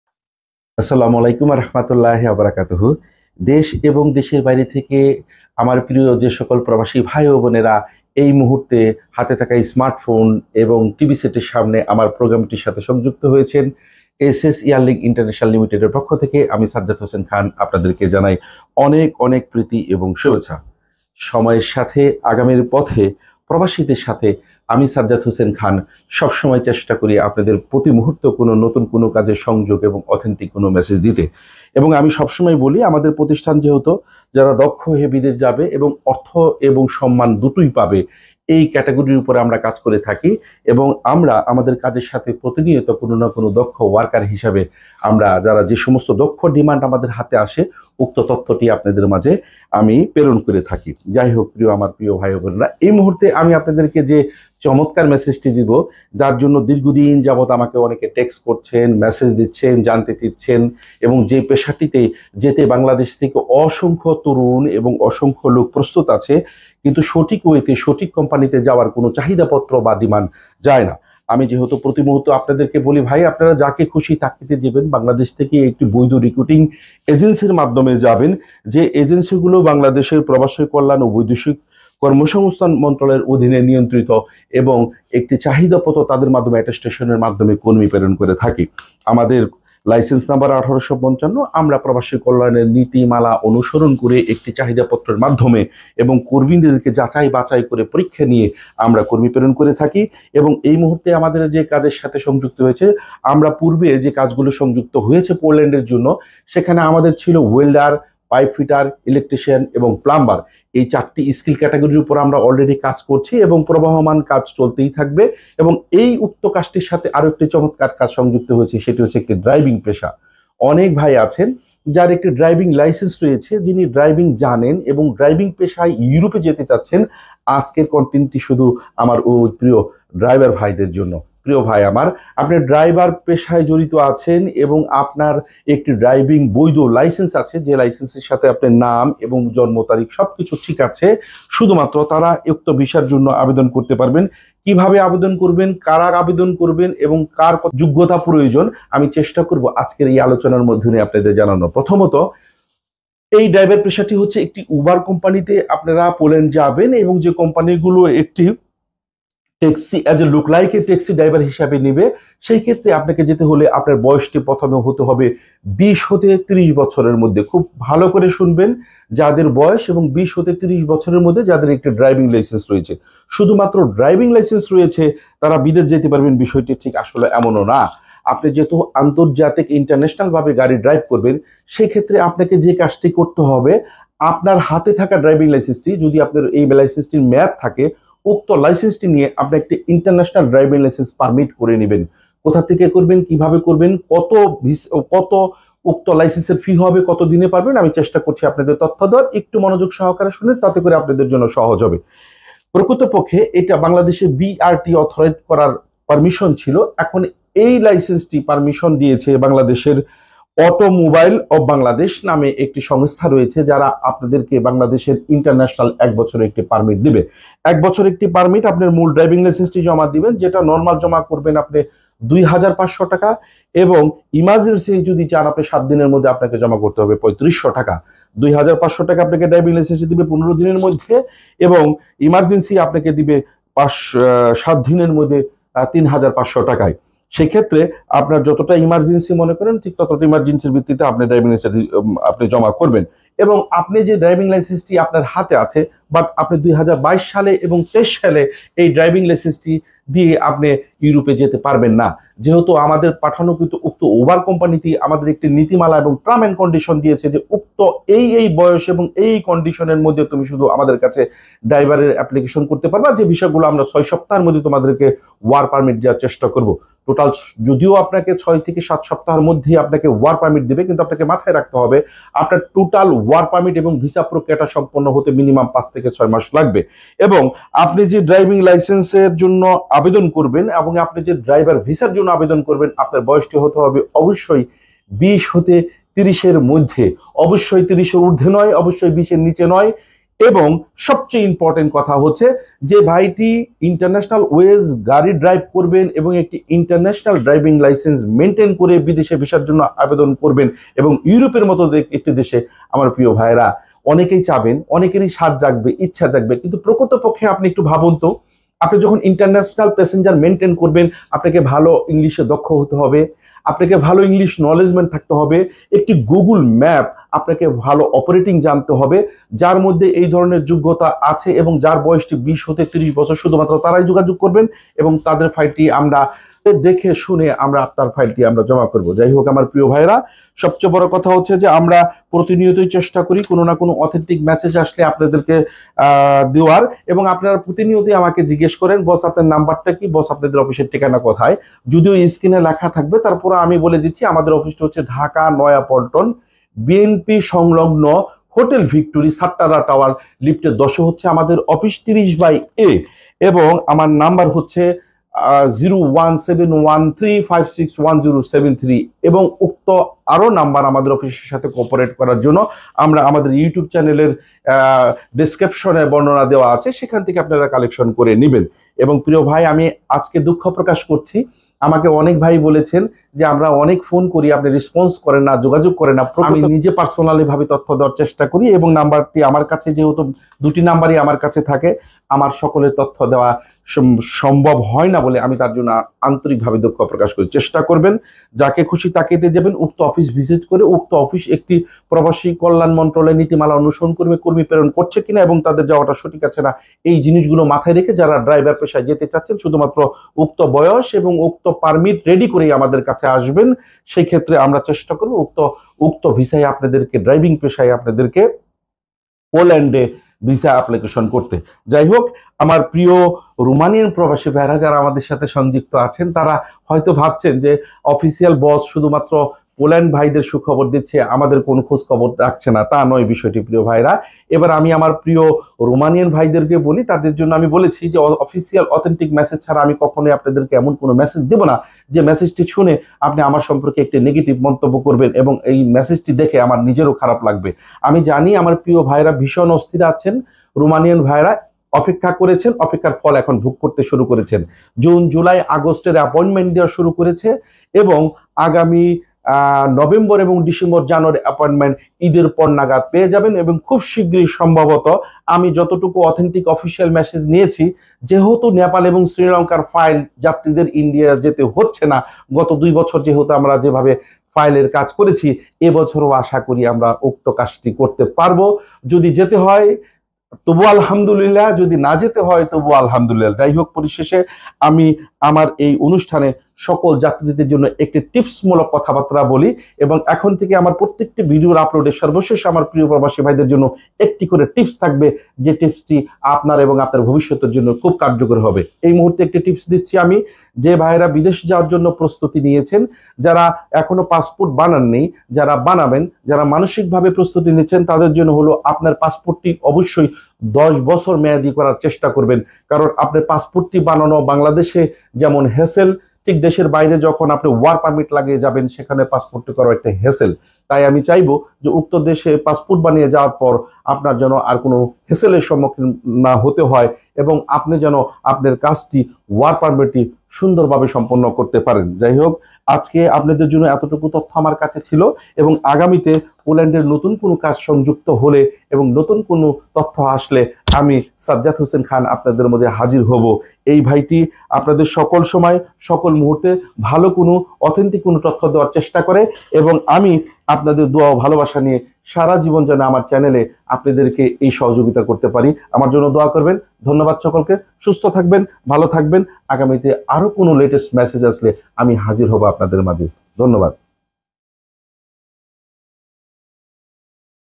পোল্যান্ড প্রবাসীদের জন্য চমৎকার সুযোগ হলো UBER ড্রাইভার ভিসায় অল্প টাকায় সল্প সময়ে পোল্যান্ড যাবার✅
sazzad vlog
অফিস ঠিকানা :- ৩০/এ সাত্তারা সেন্টার ( ১০ম তলা ) নয়াপল্টন , ঢাকা - ১০০০